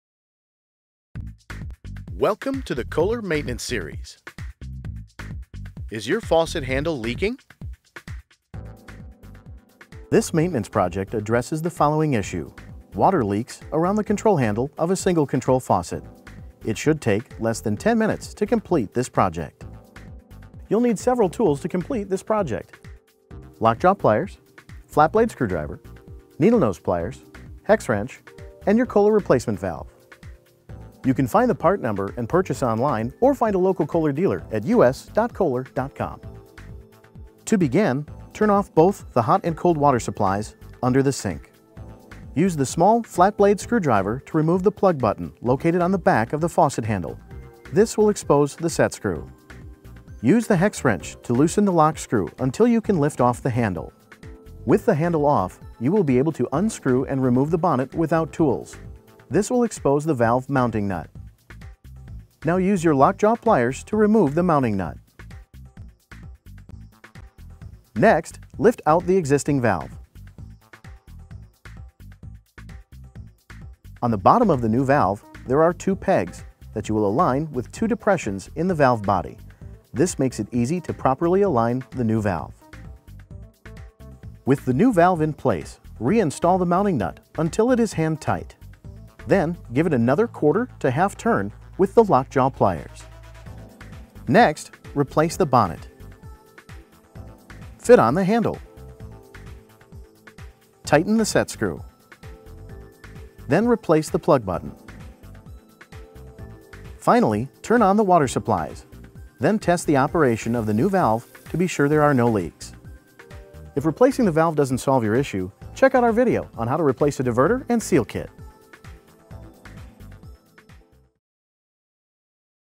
Stop Faucet Handle Leaking - How to Replace a Kitchen Faucet Valve
Is your faucet handle leaking on your single control sink faucet? See how to replace the kitchen faucet valve easily in under 10 minutes with a few tools and a Kohler replacement valve.

Looking for a new faucet? Explore KOHLER Faucets